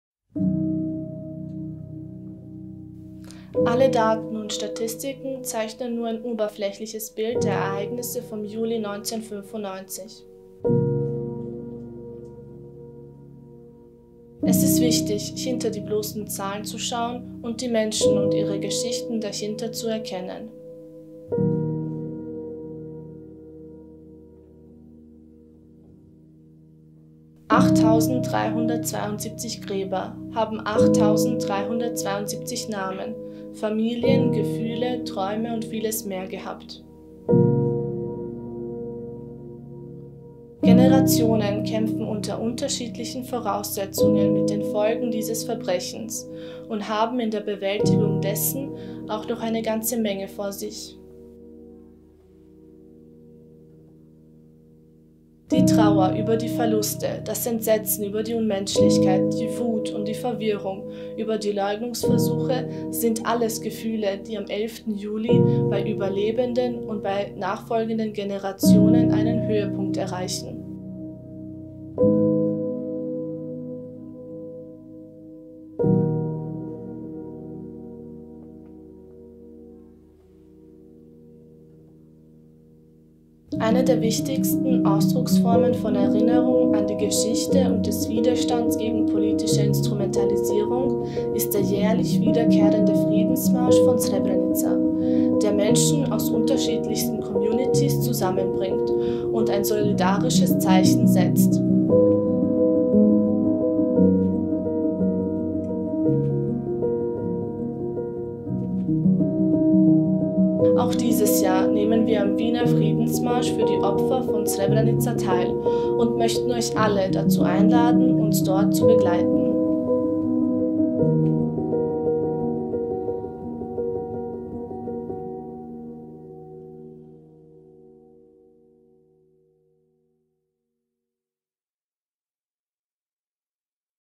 RememberingSREBRENICA
8372 Gräber - 8372 Namen, 8372 Familien, 8372 Gefühle, 8372 Träume!
Es ist wichtig, hinter die bloßen Zahlen zu schauen und die Menschen und ihre Geschichten dahinter zu erkennen.

Kurz vor dem internationalen Gedenktag des Genozids in Srebrenica möchten wir die GJM - Gemeinschaft Junger Muslim:innen zu Wort kommen lassen. Lejla gibt uns kurz vor dem 11. Juli nochmal eine Zusammenfassung der Ereignisse und teilt uns mit, wieso Srebrenica sowohl heute als auch in Zukunft ein wichtiges Thema ist und wieso es unbedingt nötig ist, die Erinnerung daran hochzuhalten.